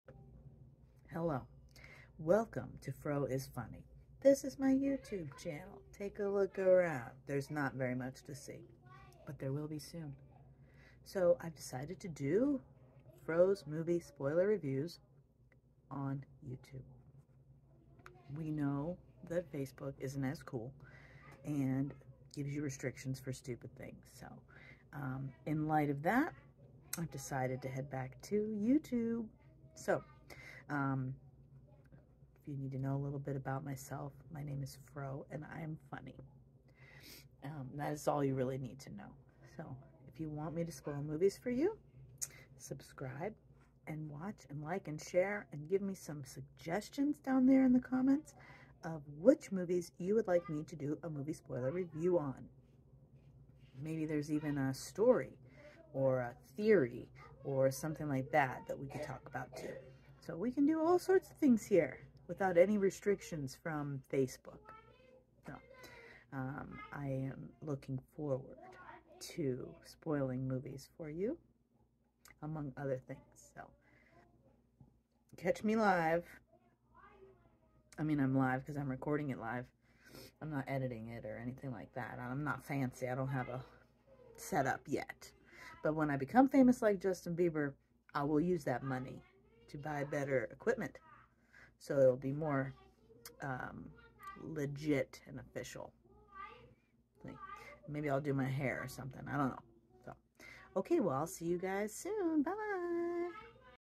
Greetings!
Hi I'm Fro! This is my channel where I spoil movies, and talk about other things that are funny! Please leave me movie suggestions in the comments! I'd love to spoil a movie for you!